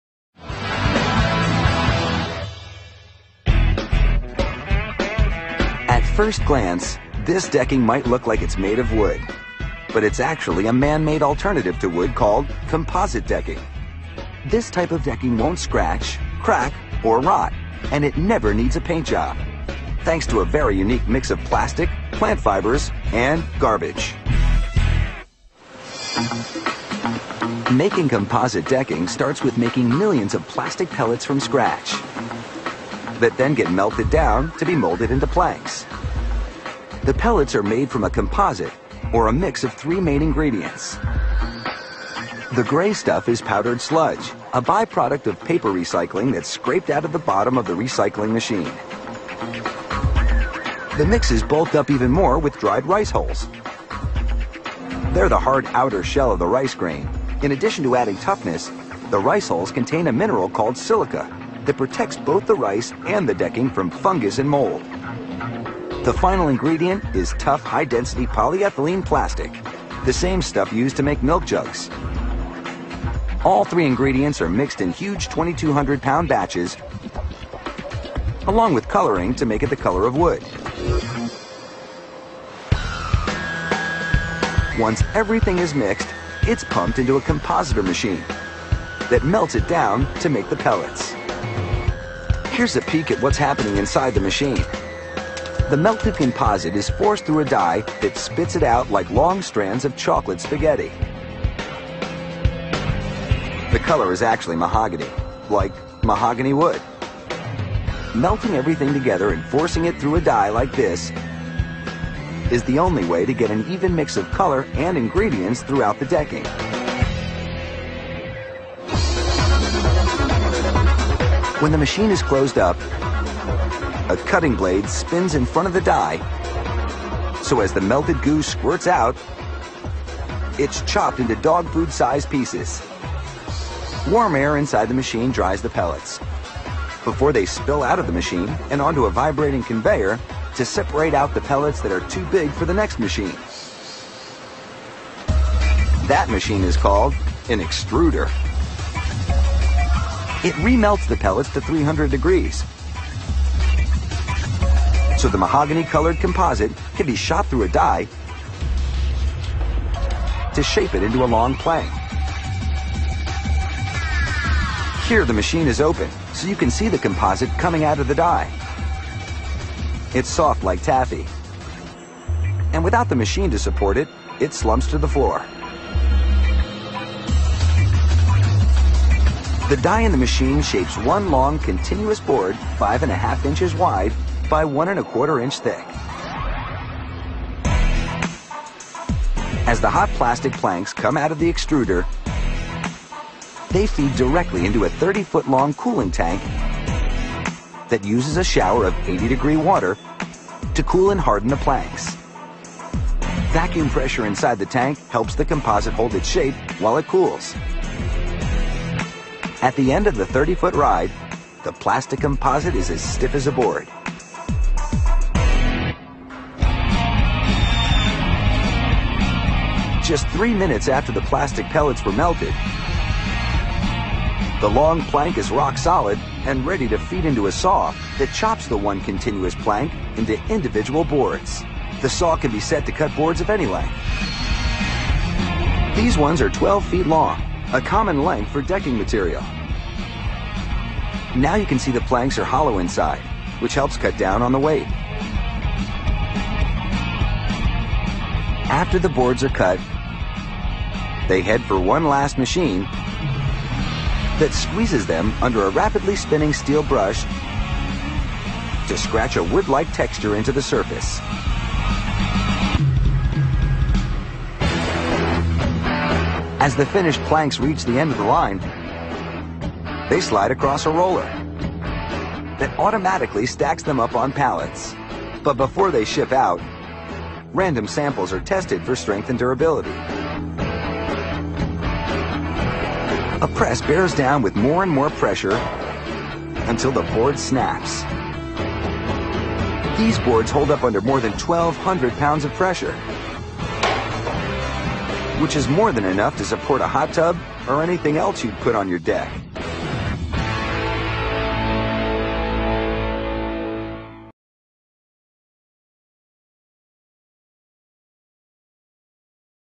Discovery Channel Tours Green Bay Decking The Science behind GeoDeck - Glenbrook U
GreenBayDecking — April 23, 2010 — Green Bay Decking opened the doors of its state-of-the-art operational facility in Green Bay, Wisconsin to the cameras of Discovery Channel for a behind-the-scenes look at how three main ingredients are used to create GeoDeck. Glenbrook Lumber has GeoDeck on display, check us out